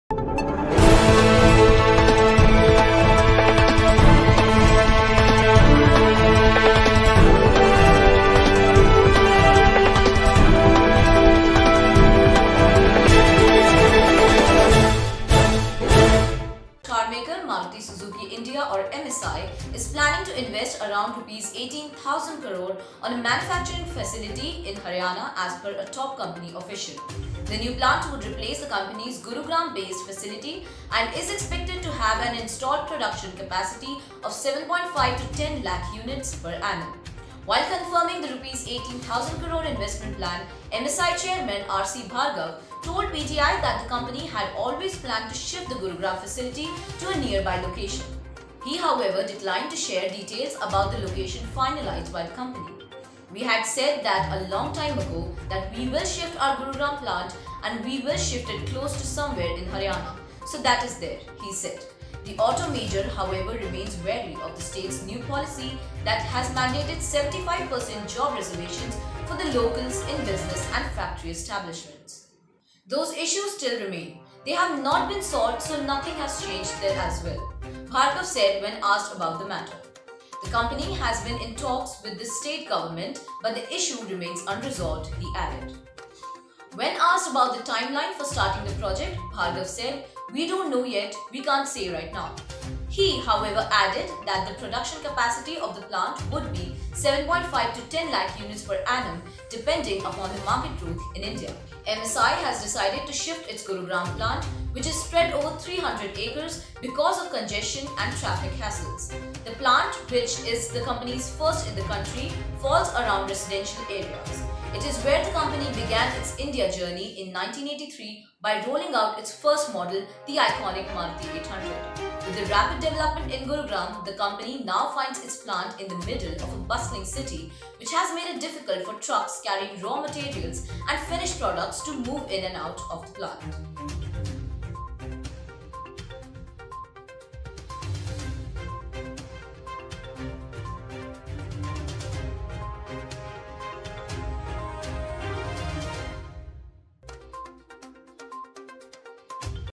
Maruti lines up Rs 18,000 cr investment for new manufacturing plant in Haryana
The country’s largest carmaker Maruti Suzuki India (MSI) is planning to invest around Rs 18,000 crore on a new manufacturing facility in Haryana, as per a top company official.

The new plant would replace the company’s Gurugram-based facility and is expected to have an installed production capacity of 7.5-10 lakh units per annum.

While confirming the Rs 18,000 crore investment plan, MSI Chairman RC Bhargava told PTI that the company had always planned to shift the Gurugram facility to a nearby location.

Anchor: Sakshi

Reporter: Ritika

Video Editor: Kushank